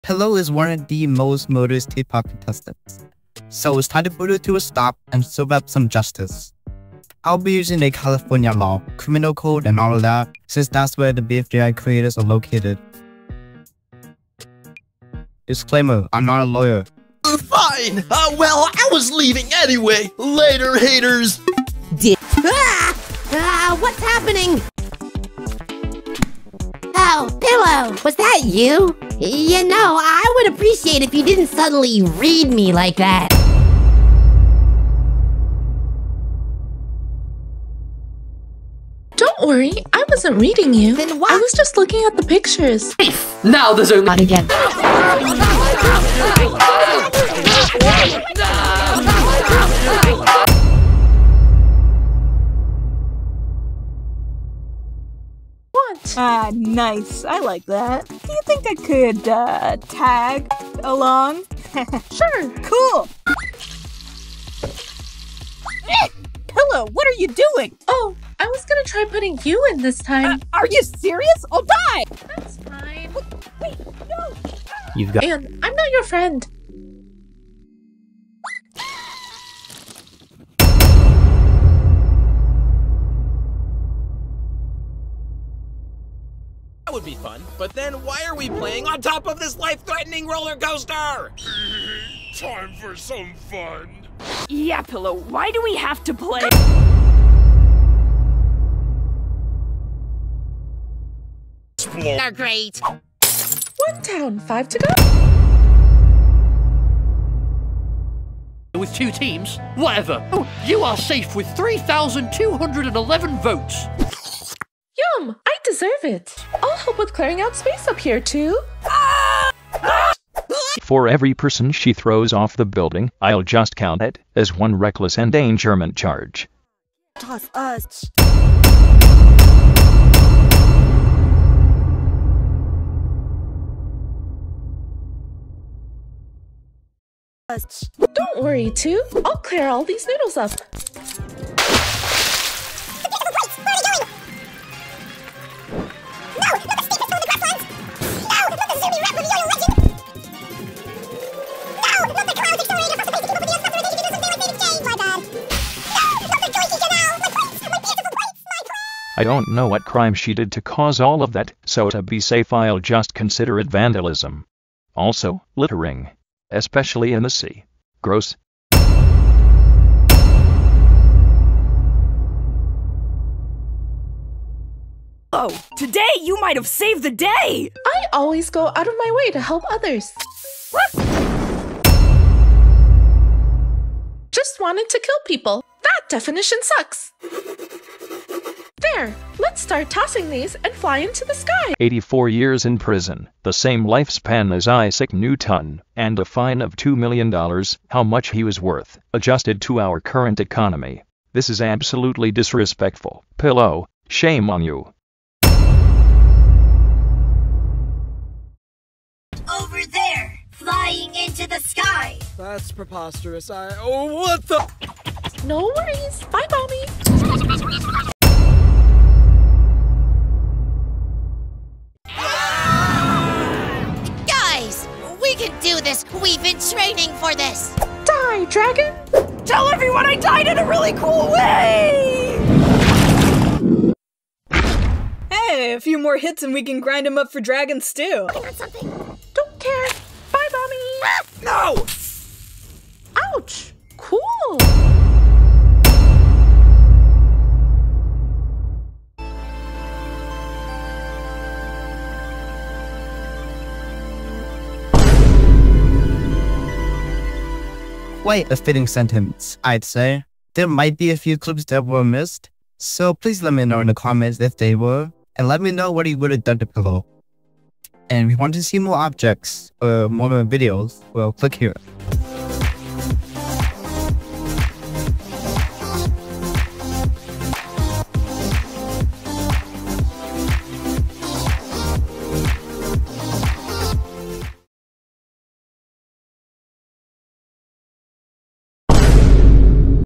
What If Pillow Was Charged For Her Crimes?
What would Pillow from BFDI (TPOT) be charged with? She is one of the most dangerous contestants - so let's charge her!

Thanks @YellowScarf_itsyellow  for this video idea!
I'm not a lawyer - this video is a simulation of what would happen in legal proceedings, but it is not meant to be as accurate or realistic as possible. Didn't really follow the format here, but this is a case. I voice acted my voice combined with the AI model. No TTS is used.

Inspired by CorpsePrime